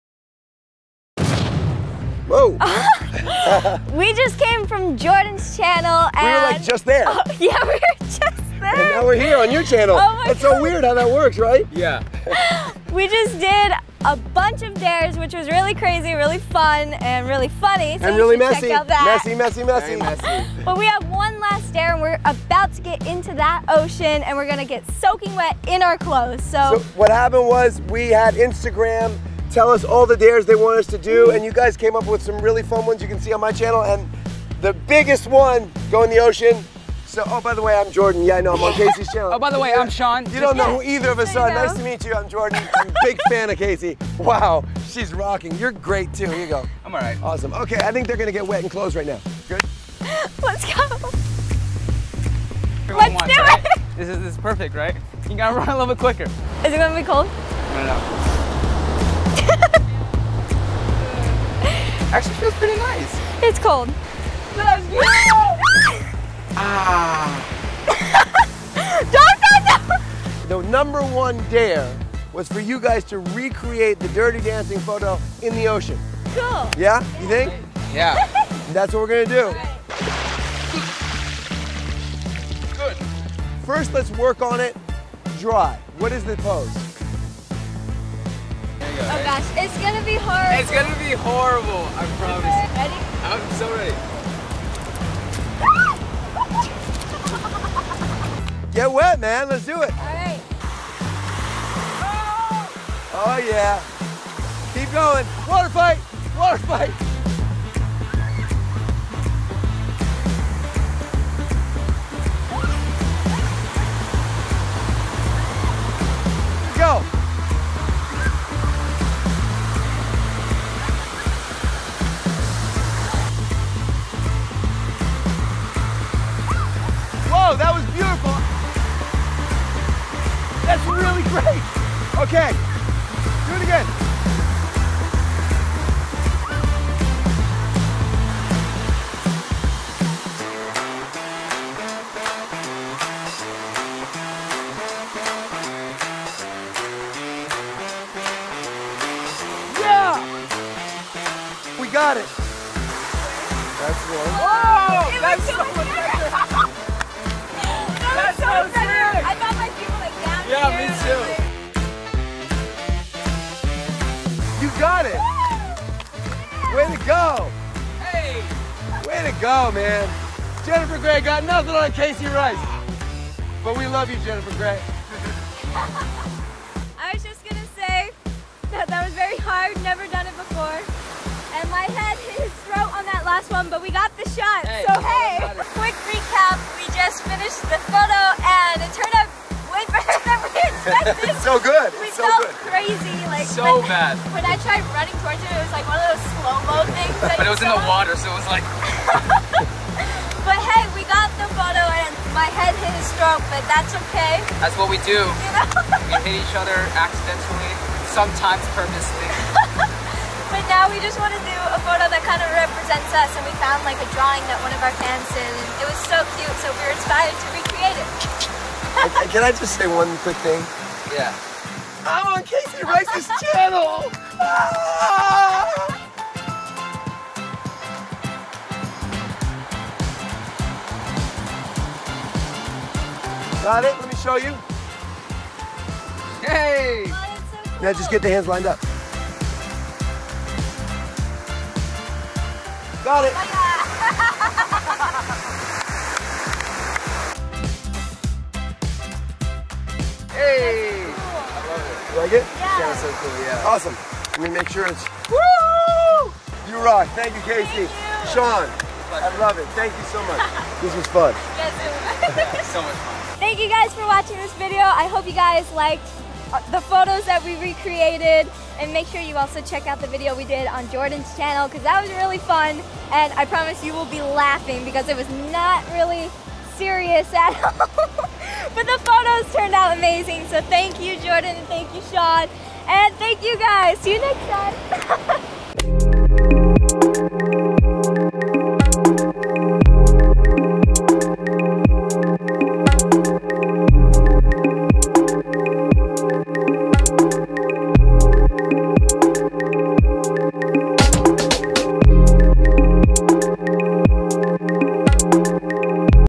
Sean and Kaycee X Jordan Matter | THE FINAL DARE - Kaycee Rice
Hope you enjoyed our FINAL DARE at Venice Beach! Thank you guys for helping us out on what to do for our photoshoot. Thank you @JordanMatter for making this possible. It was a joy to work with you. Thank you Sean for doing this with me. And thank you to IG: @seaycee_art for the adorable drawing of Sean and I. It represents us so well!